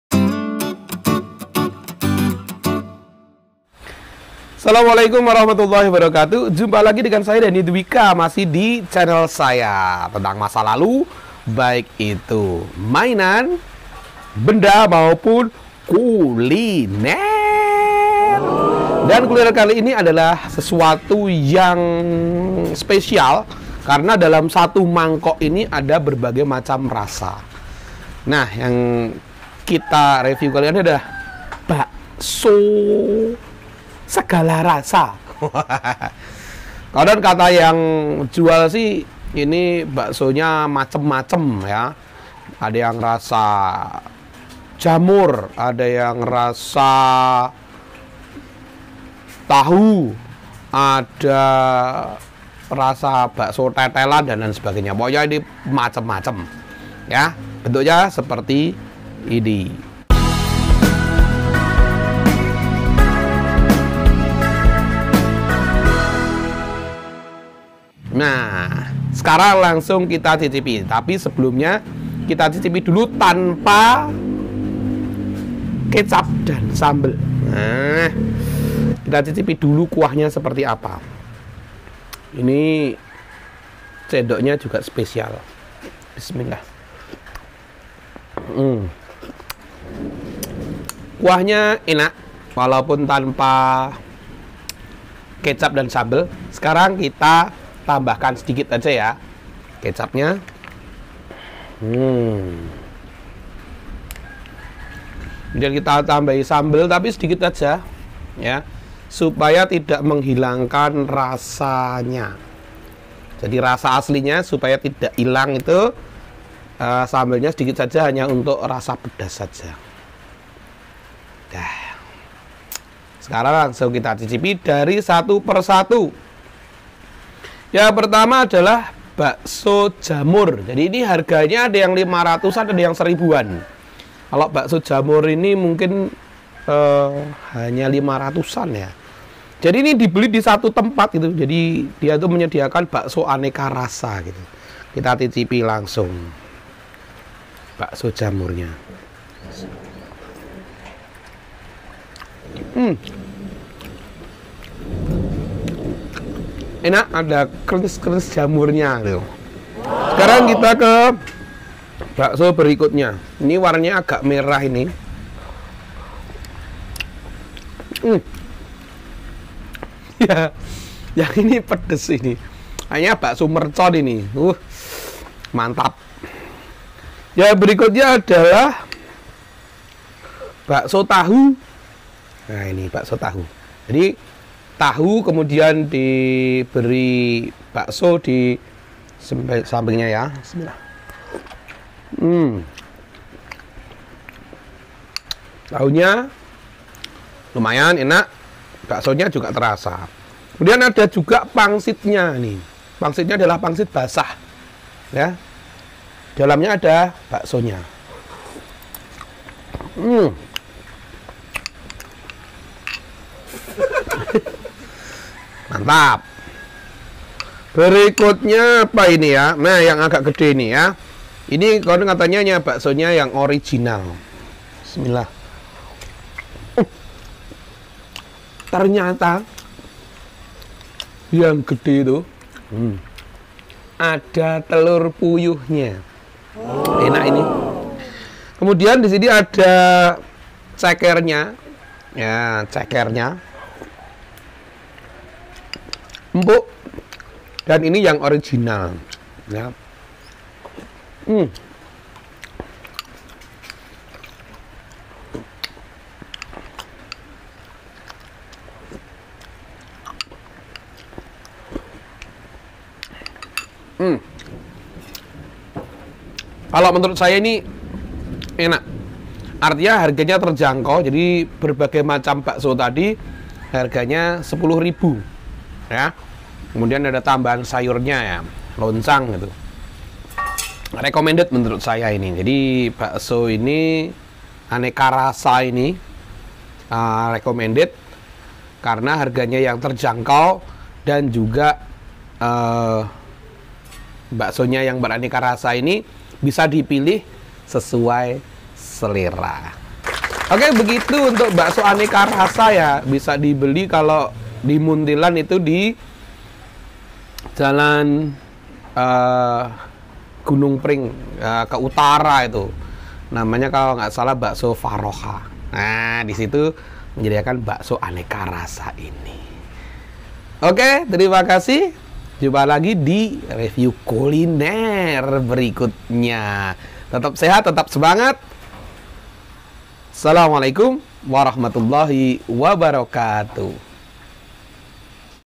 Bakso aneka rasa/kuliner unik/inspirasi usaha/Kuliner Indonesia
Bakso adalah kuliner yang sudah dikenal sejak dulu. Dulu, bakso hanya terbuat dari bahan daging sapi. tapi kini, seiring perkembangan zaman, bakso pun dimodifikasi. Aneka rasa menjadi saya tarik bakso ini. Bakso jamur, bakso tahu, pedes, dll menjadikan bakso ini cukup digemari karena pembeli dapat memilih bakso sesuai selera. Harganya pun terjangkau. Mulai 500-2000 rupiah saja.